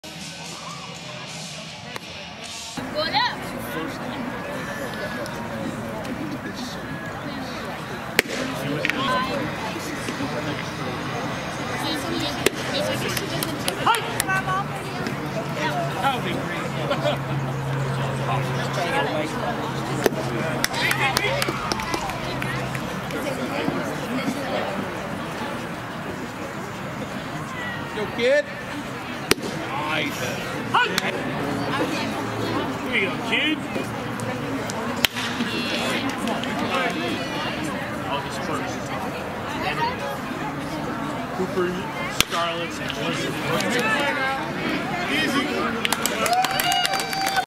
Billy Layne Jr. Pitching For Somerset Patriots
Somerset Patriots pitcher Billy Layne Jr. throws a perfect inning against the New Jersey Blasters